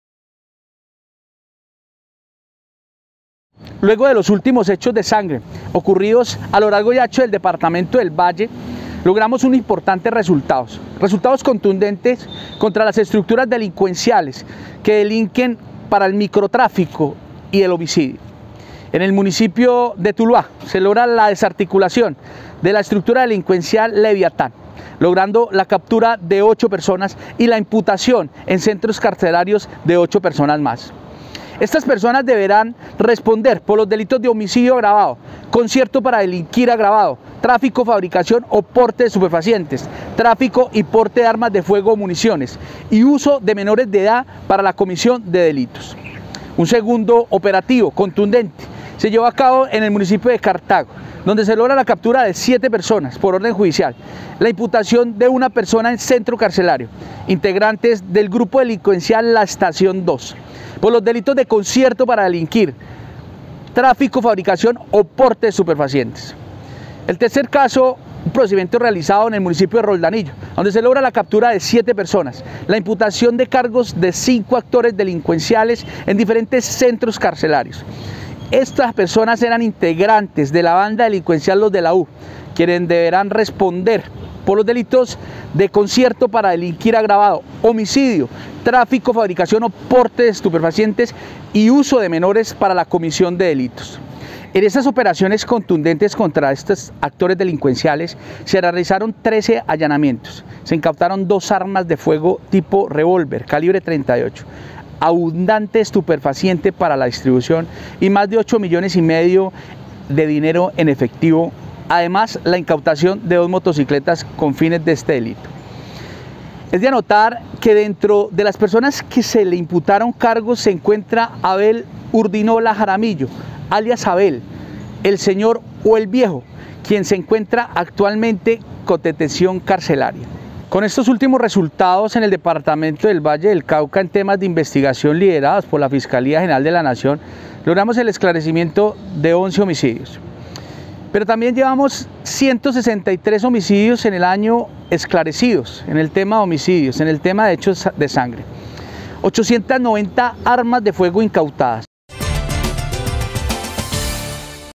22 personas capturadas y 14 imputaciones de cargos en centros carcelarios.
Fuerte redada contra la criminalidad en el Valle del Cauca.
 22 personas capturadas y 14 imputaciones de cargos en centros carcelarios.

Esclarecemos 11 homicidios presentados en el año 2020 y lo corrido del presente año.

Mediante acciones a la estrategia 24/9, el departamento de Policía Valle en acciones de reducción del delito, logra en las últimas horas y de manera simultánea la captura de 22 personas por orden judicial, además 14 imputaciones en centros carcelarios de tres bandas delincuenciales en los municipios de Tuluá, Cartago y Roldanillo.

En el municipio de Tuluá se desarticula la banda delincuencial denominada “Leviatán” donde se capturan 08 presuntos delincuentes, además se imputan cargos a 08 personas más en centros carcelarios, quienes deberán responder por los delitos de Homicidio agravado; Concierto para delinquir agravado; Tráfico, fabricación o porte de estupefacientes; Fabricación, tráfico y porte de armas de fuego o municiones; Uso de menores de edad para la comisión de delitos.

Es de anotar  que se esclarecen 11 casos de homicidios presentados en la jurisdicción del municipio de Tuluá en los años 2020 y 2021.

Cabe recalcar que esta banda delincuencial tenia como injerencia criminal los barrios La Paz, Invasión Brisas del Rio, Corregimiento de Agua Clara, Urbanización San Francisco, Galería e invasión la Playita, Bosques de Maracaibo, la Inmaculada, San Antonio y Chiminangos.

En el segundo operativo llevado a cabo en el municipio de Cartago se capturan 7 personas por Orden Judicial y 01 Imputación en centro carcelario, integrantes del grupo delincuencial "La Estación II", por los delitos de Concierto para Delinquir Agravado y Tráfico, Fabricación o Porte de Estupefacientes.

Cabe recalcar que el operativo se llevó a cabo en el barrio Jorge Eliecer Gaitán sector Parque La Estación Comuna 1, donde este grupo delincuencial trabajaba en la modalidad de Outsourcing en la línea de microtráfico de la estructura del GDO "Los Flacos", siendo dinamizadores de homicidios y tráfico de estupefacientes.

En el tercer procedimiento llevado a cabo en el municipio de Roldanillo se capturan 7 personas y la imputación de cargos a 5 actores delincuenciales desde diferentes centros penitenciarios, integrantes de la banda delincuencial “Los de la U”, quienes deberán responder por los delitos de Concierto para delinquir agravado, homicidio, Trafico, fabricación o porte de estupefacientes y uso de menores para la comisión del delito.

En este operativo se materializaron 05 Allanamientos, incautando 01 un arma de fuego tipo revolver calibre 38, abundante estupefaciente listo para su distribución, 03 celulares, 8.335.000 dinero en efectivo, además, la incautación de 02 motocicletas con fines de comiso.

Es de anotar que dentro de las personas que se le imputaron cargos se encuentra Abel Urdinola Jaramillo alias “Abel, El Seño o El Viejo” quien se encuentran actualmente con detención carcelaria.